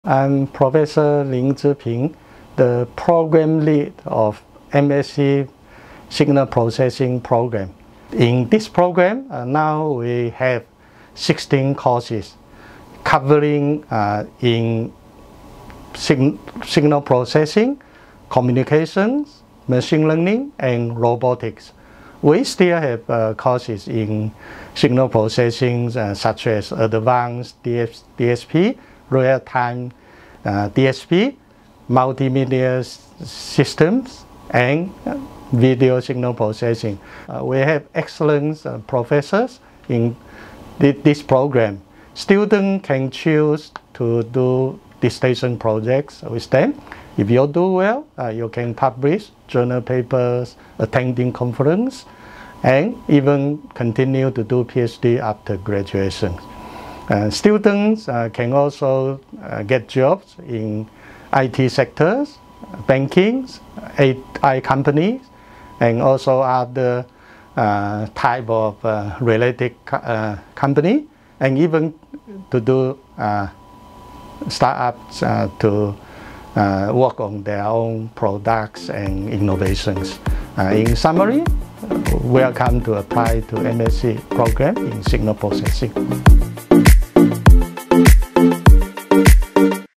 Assoc Prof Lin Zhiping (Programme Director) for MSc (Signal Processing & Machine Learning) - NTU EEE
The MSc (Signal Processing and Machine Learning) programme is designed for practicing engineers, hardware and software designers, R & D managers, and industry planners who seek an understanding of current approaches and evolving directions for DSP technologies. It is also intended for engineers who anticipate future involvement in this area.